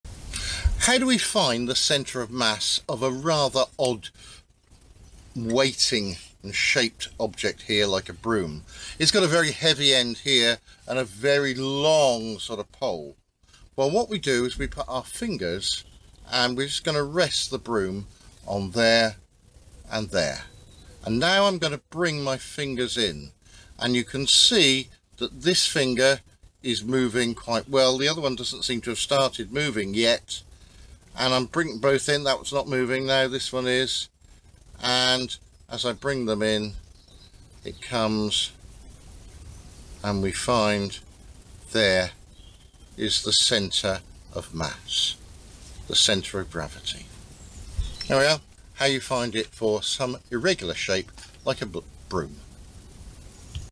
How to find centre of mass of an object
Looking at a broom to find the centre of mass. Its a simple trick to do. Hold out the broom horizontally balanced on two fingers. Move the hands together slowly and often you find one hand moves and the other doesn't when the hands meet - that's the centre of mass.
We find the centre of mass to help balance things like getting a picture straight on a wall and we use this idea of centre of mass to stop things blowing over because they are "top heavy".